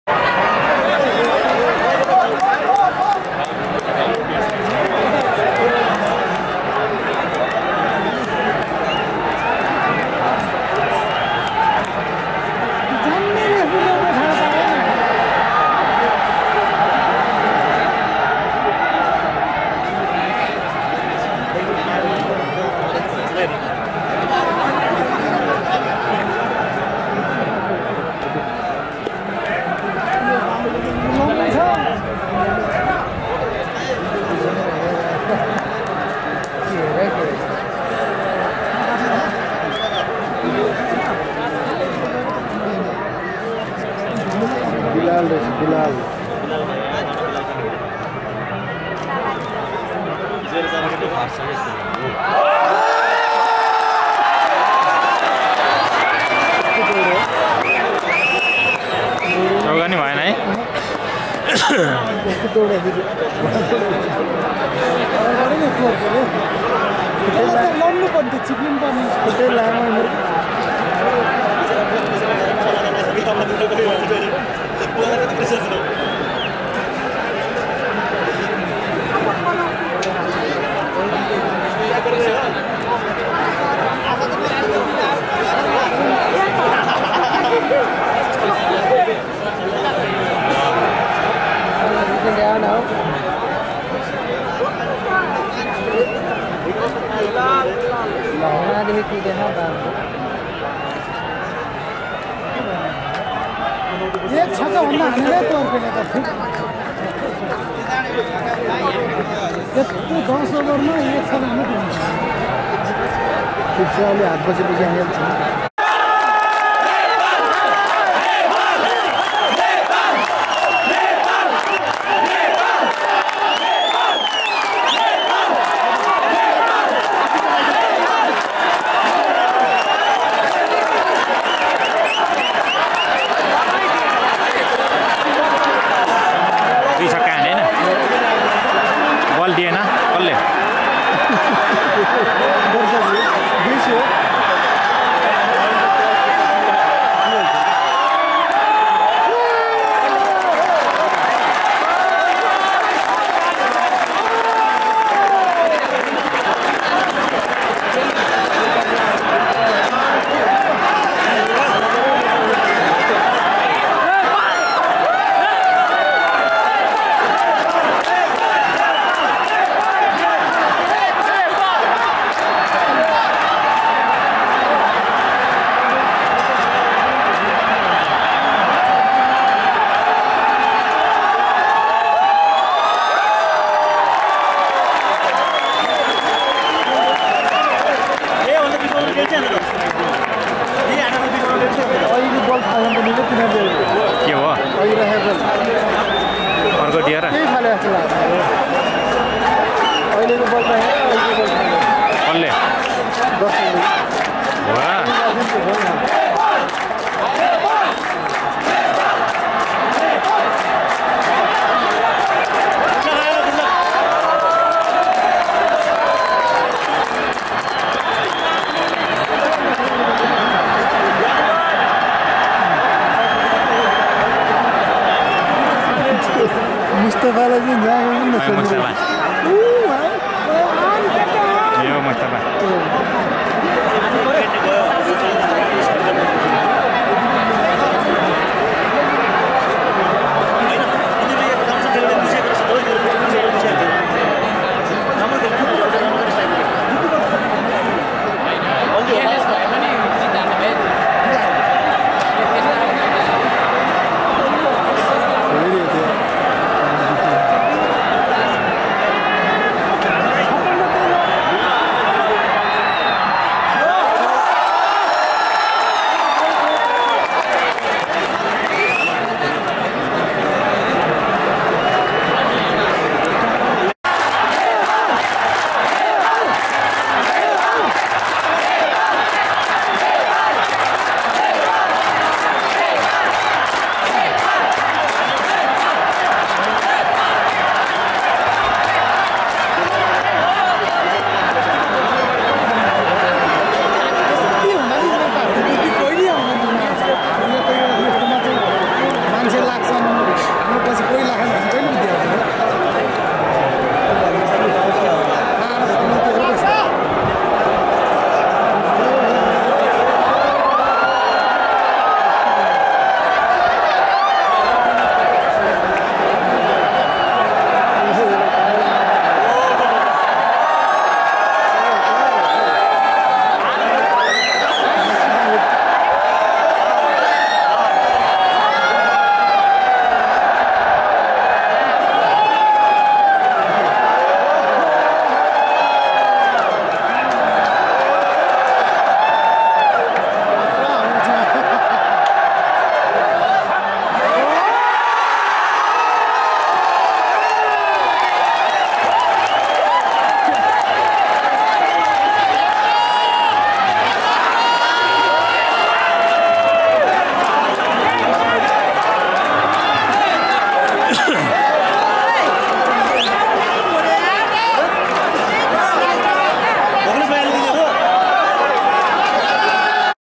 Record Number and Noise in Tribhuvan University Cricket Stadium | Mexican Wave in the ground
Location Koteshwor
Copyright@AdamVlog